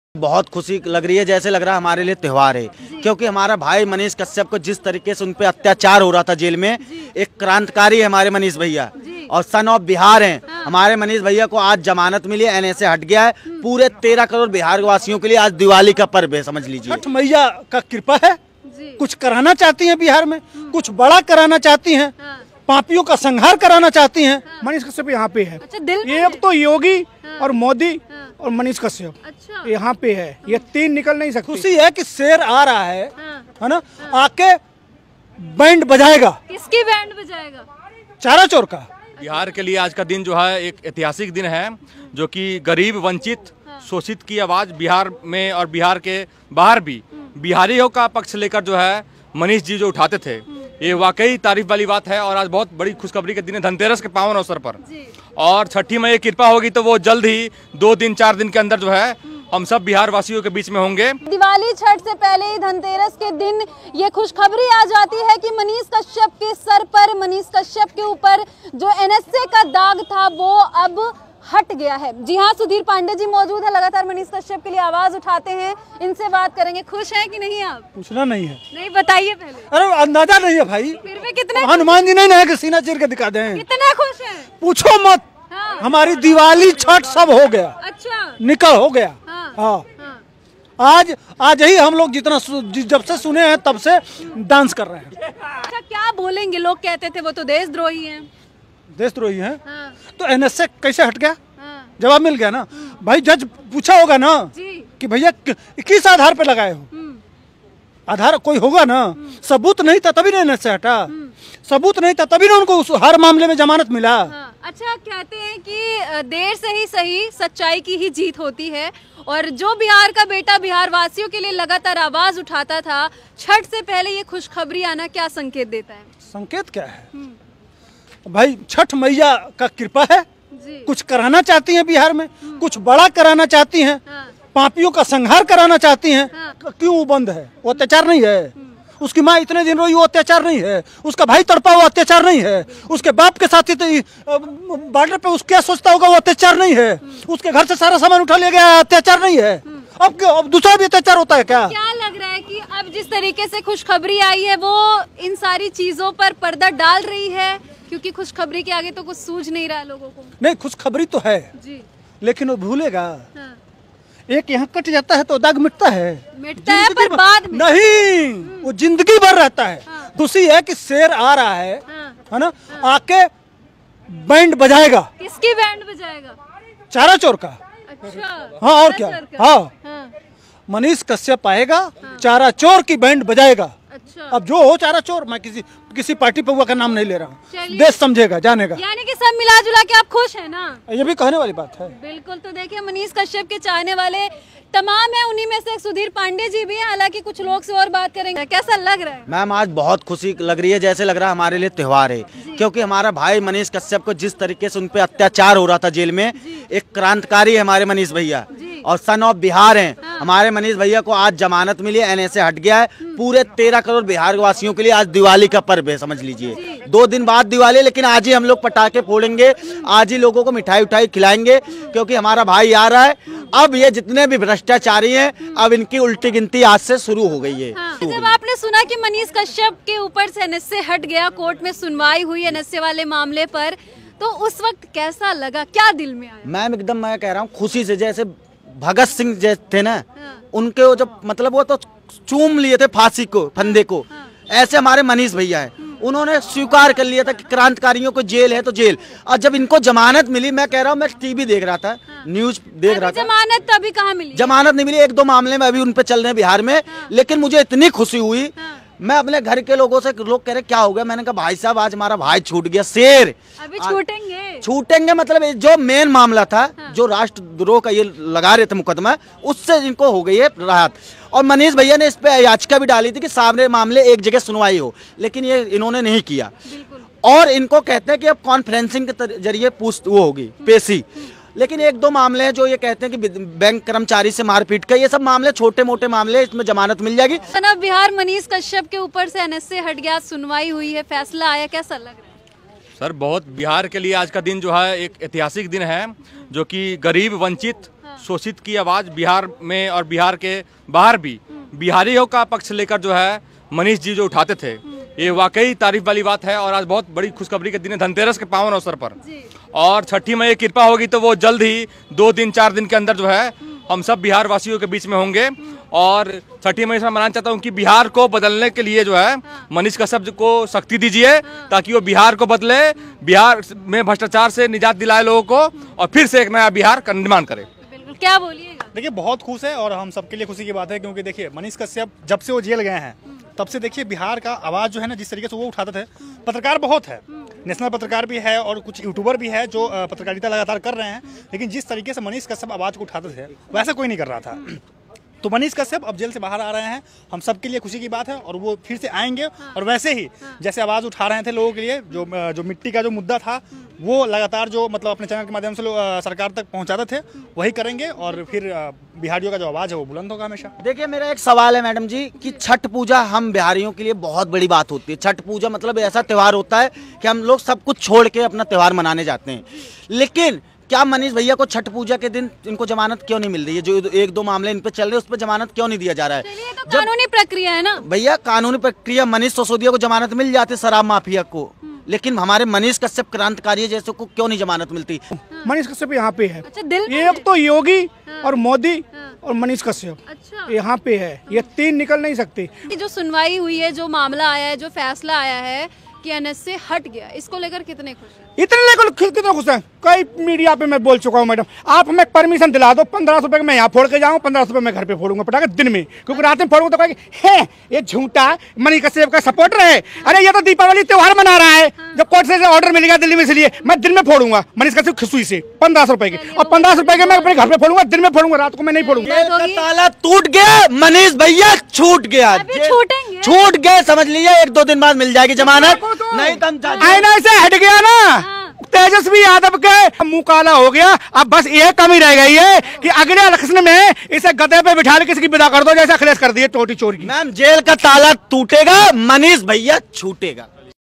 मनीष कश्यप के ऊपर से NSA हटने पर लोगों में खुशी का माहौल, देखिये क्या बोल रहे लोग ।।ManishKashyap
मनीष कश्यप के ऊपर से NSA हटने पर लोगों में खुशी का माहौल, देखिये क्या बोल रहे लोग ।।